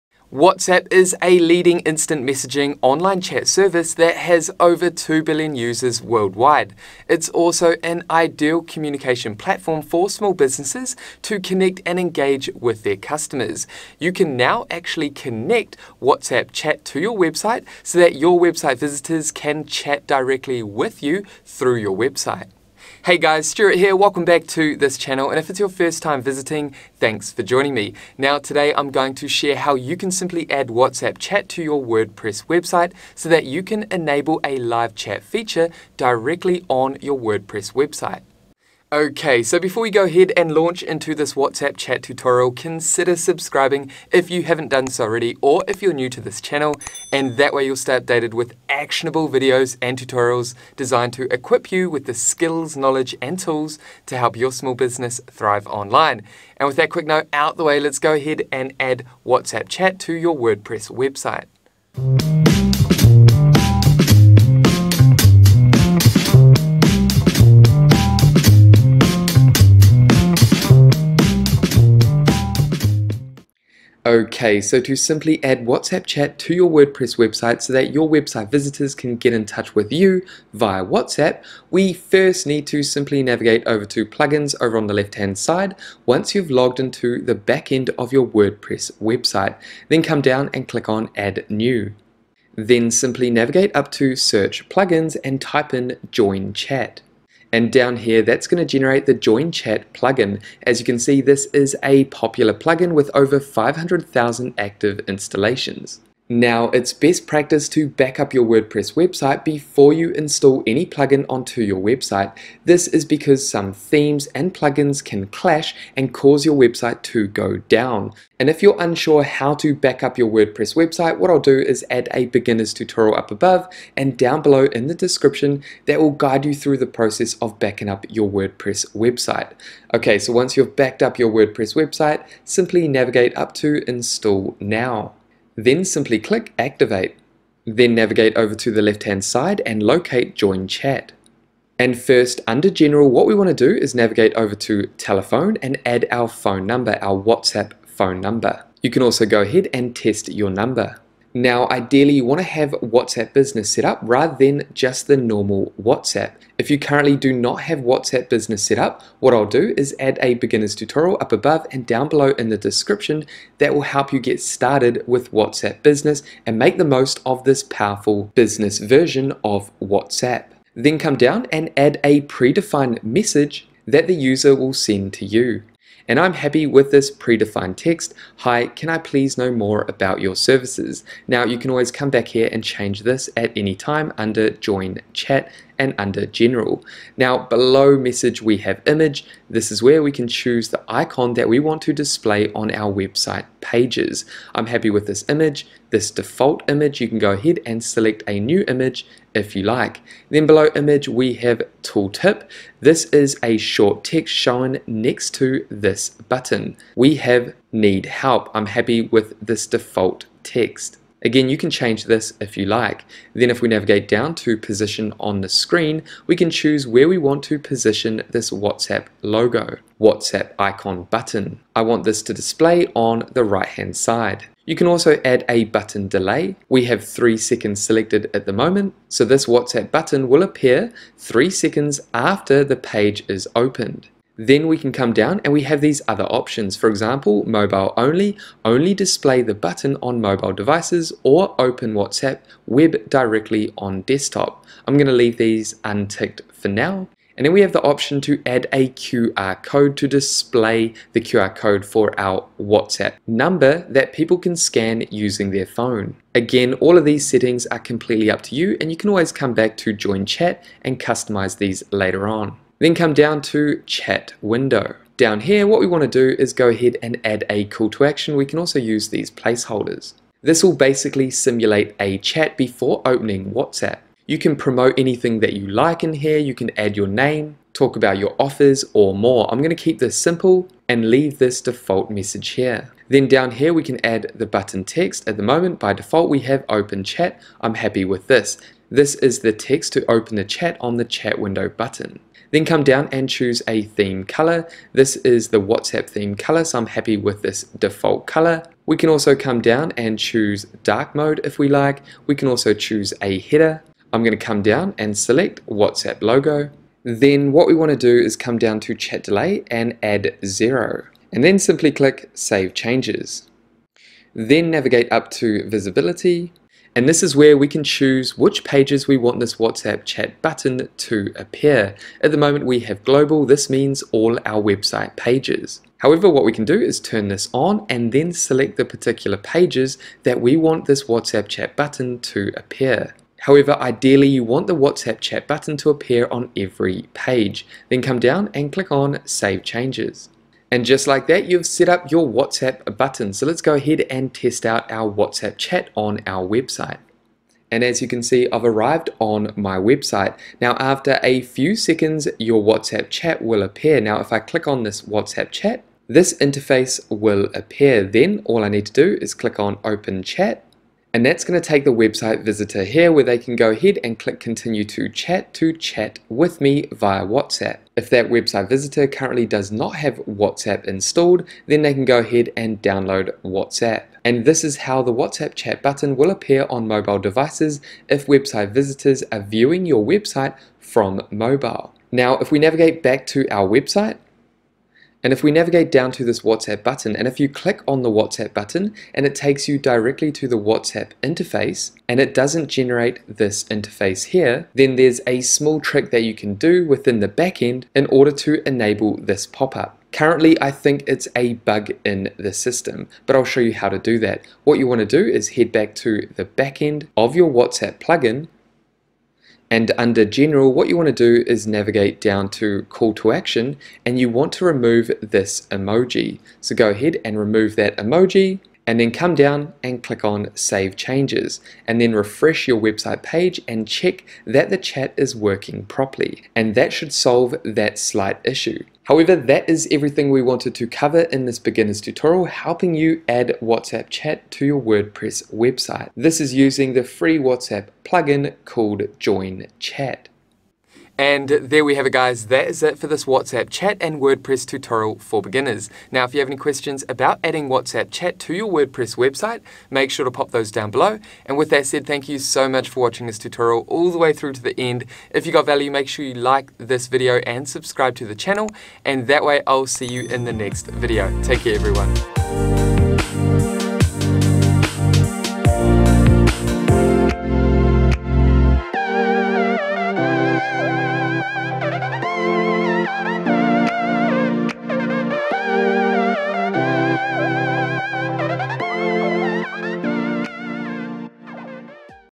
How To Add WhatsApp Chat To WordPress | Free WhatsApp Chat Plugin (2023)
Today, I dive into this WhatsApp chat tutorial for WordPress and share how to add WhatsApp chat to WordPress using a free WhatsApp chat plugin.

WhatsApp is a leading online chat service built for individuals and businesses with over 2 billion users! WhatsApp can simply be embedded onto your WordPress website so that your website visitors can chat with you through WhatsApp.

► Today we navigate through the below chapters for this WhatsApp chat tutorial:

0:00 Intro
01:16 Getting started
01:40 Install the free Join chat plugin
02:24 Join chat settings
05:52 Test WhatsApp Button
06:27 Chat Issue Solution
07:22 Outro

Let me know if you found this WhatsApp chat tutorial helpful. Also, if you require any help or support, make sure to get in touch with us today.